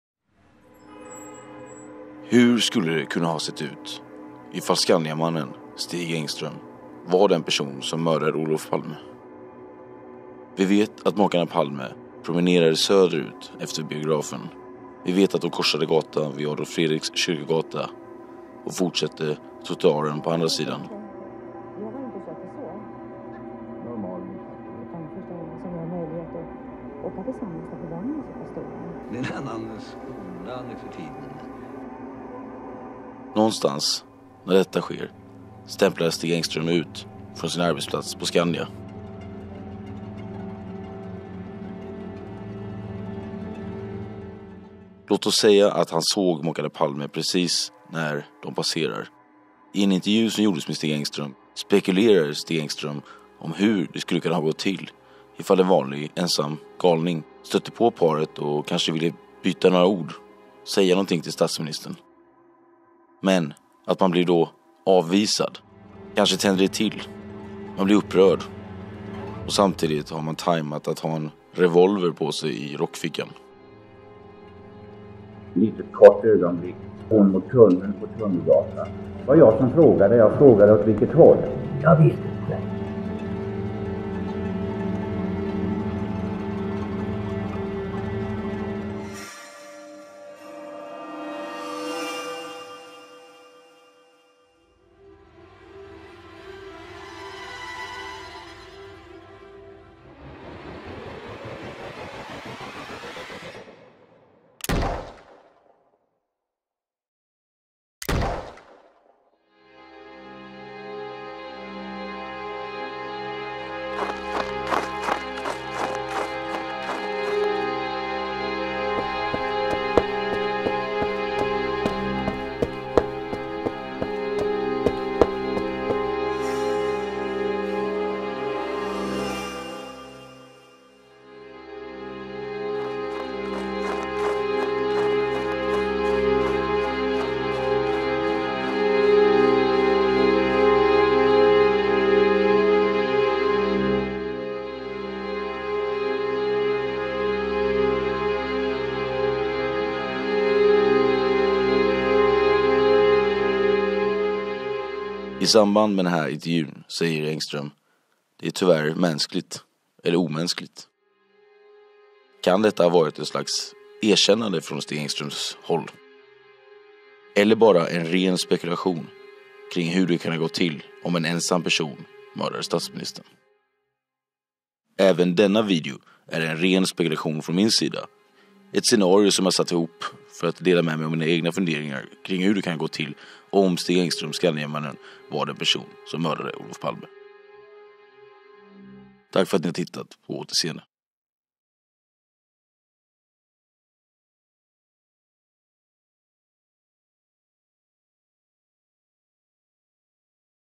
Så här kan STIG ENGSTRÖM mördat OLOF PALME
Jag har satt ihop ett scenario om hur mordet kan ha gått till – om det nu utfördes av Skandiamannen. Scenariot grundar jag på det sista uttalande som Skandiamannen gjorde till journalisten Jan Arvidsson i en intervju.